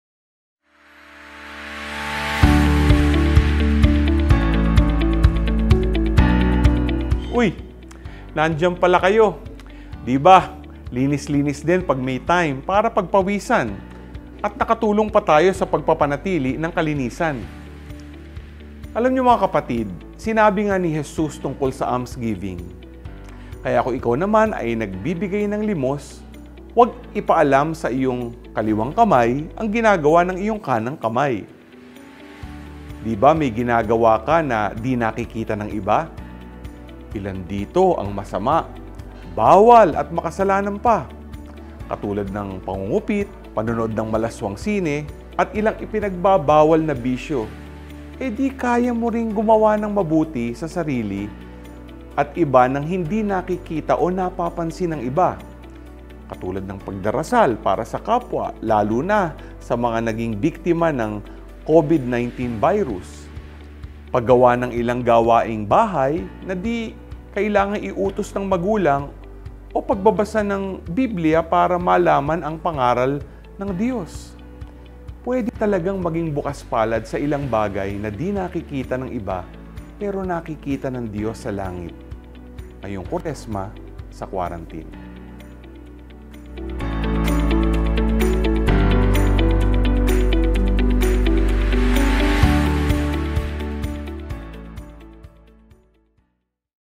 Kuwaresma sa Quarantine Ep. 9
Pagbibigay, kailangan pa bang makita ng iba?
Samahan si Bishop Mylo Hubert magnilay para maging
makabuluhan ang iyong Kuwaresma sa Quarantine.